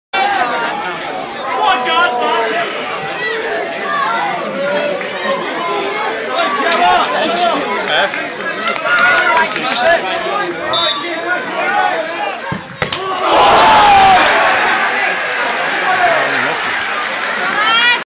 PENALTY SAVE! Witton 1 FC United of Manchester 1
Great penalty save by former Northwich Victoria 'keeper James Spencer. 27/08/12